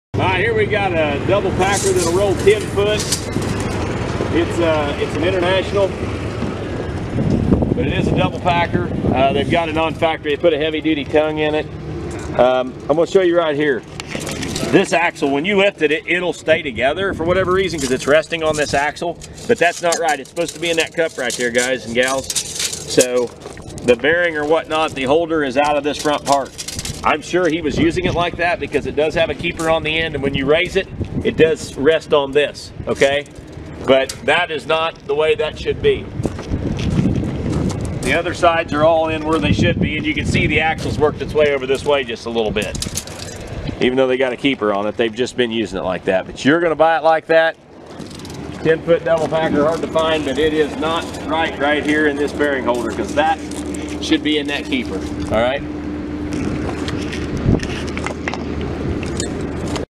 INTERNATIONAL 10 For Sale
International 10ft Double Packer- Non Factory Heavy Duty Tongue, Bearing Not In Cup Right,   *See Pics & Video For More Details *Sells Absolute!  ~Please Read Terms & Conditions Before Bidding